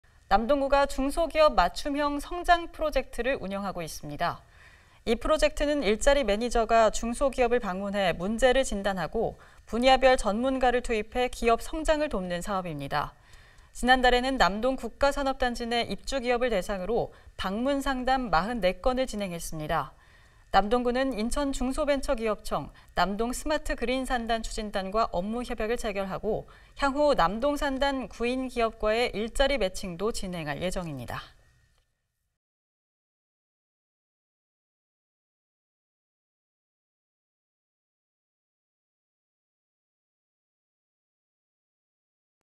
남동구, 중소기업 맞춤형 성장 프로젝트 운영
[기사내용]
남동구가
중소기업 맞춤형 성장 프로젝트를 운영하고 있습니다.

이 프로젝트는
일자리 매니저가 중소기업을 방문해 문제를 진단하고,
분야별 전문가를 투입해 기업 성장을 돕는 사업입니다.

지난달에는 남동국가산업단지 내 입주기업을 대상으로
방문상담 44건을 진행했습니다.

남동구는 인천중소벤처기업청, 남동스마트그린산단추진단과 업무협약을 체결하고
향후 남동산단 구인기업과의 일자리 매칭도 진행할 예정입니다.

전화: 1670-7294